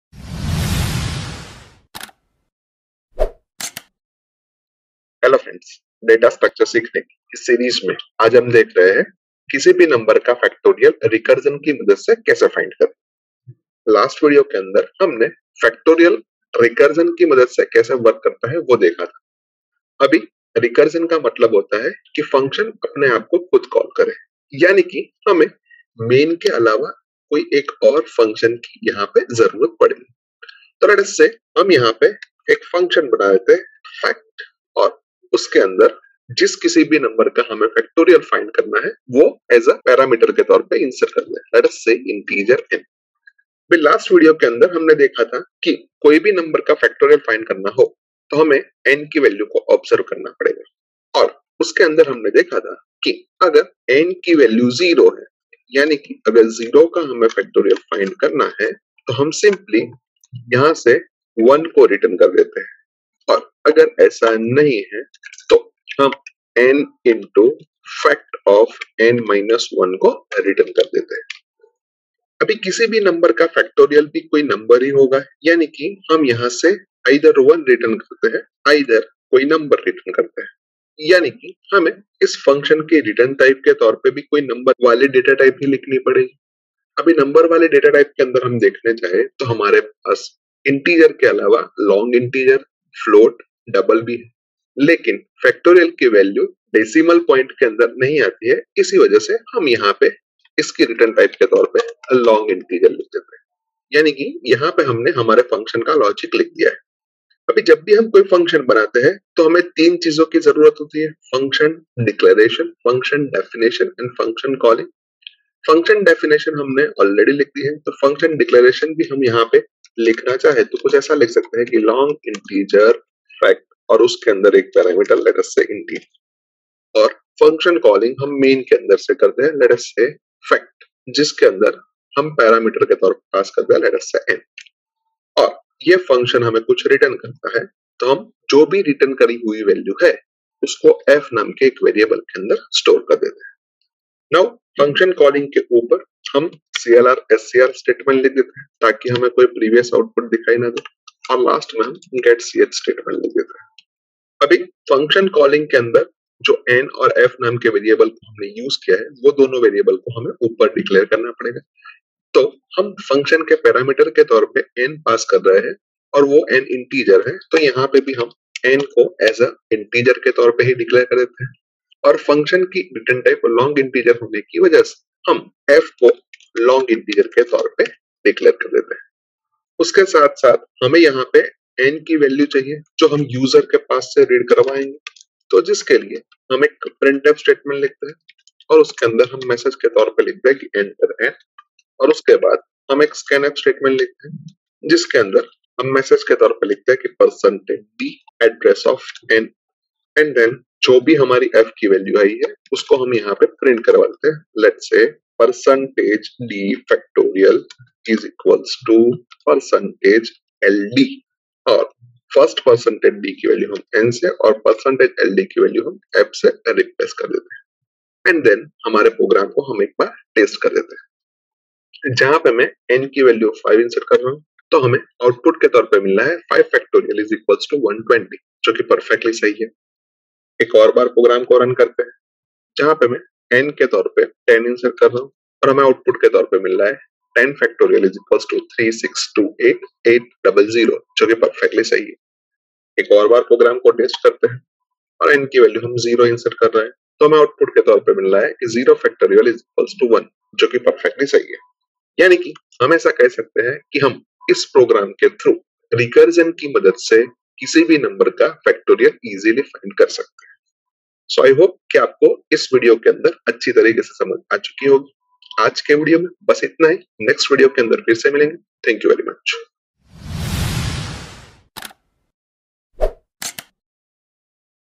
Program to Find Factorial of a Number using Recursion [Hindi] | Kaushal Madhu
52nd video on DataStructures

In this video, I am talking ​about:
- Program to find factorial of a number using recursion with example